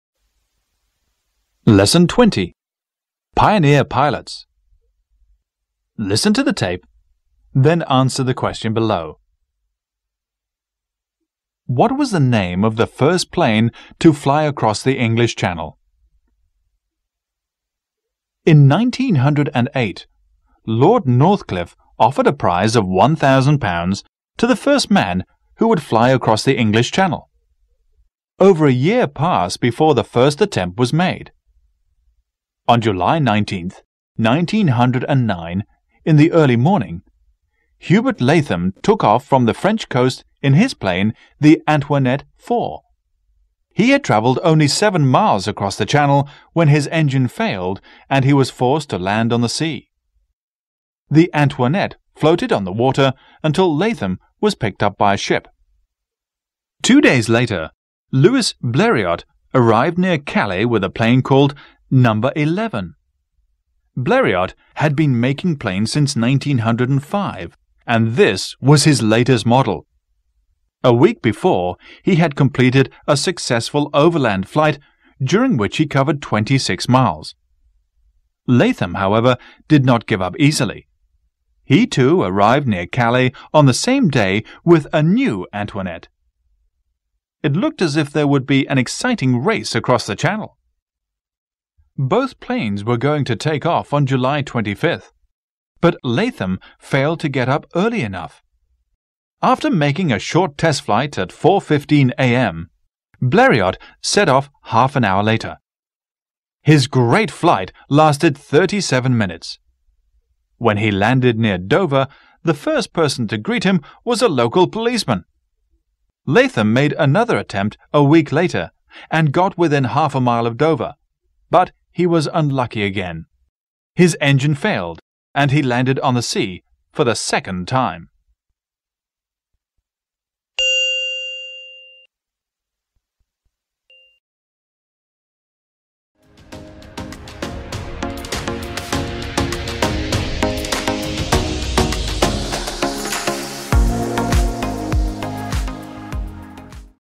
PIONEER PILOTS - NEW CONCEPT ENGLISH 3 - DEVELOPING SKILLS
New Concept English 3 is a series of L.G. Alexander books that cool

In 1908 Lord Northcliffe offered a prize of $1,000 to the first man who would fly across the English Channel. Over a year passed before the first attempt was made. On July 19th, 1909, in the early morning, Hubert Latham took off from the French coast in his plane the 'Antoinette IV.' He had travelled only seven miles across the Channel when his engine failed and he was forced to land on sea. The 'Antoinette' floated on the water until Latham was picked up by a ship.
    Two days alter, Louis Bleriot arrived near Calais with a plane called 'No. XI'. Bleriot had been making planes since 1905 and this was his lattes model. A week before, he had completed a successful overland flight during which he covered twenty-six miles. Latham, however, did not give up easily. He, too, arrived near Calais on the same day with a new 'Antoinette'. It looked as if there would be an exciting race across the Channel. Both planes were going to take off on July 25th, but Latham failed to get up early enough, After making a short test flight at 4,15 a.m., Bleriot set off half an hour later. His great flight lasted thirty-seven minutes. When he landed near Dover, the first person to greet him was a local policeman. Latham made another attempt a week later and got within half a mile of Dover, but he was unlucky again. His engine failed and he landed on the sea for the second time.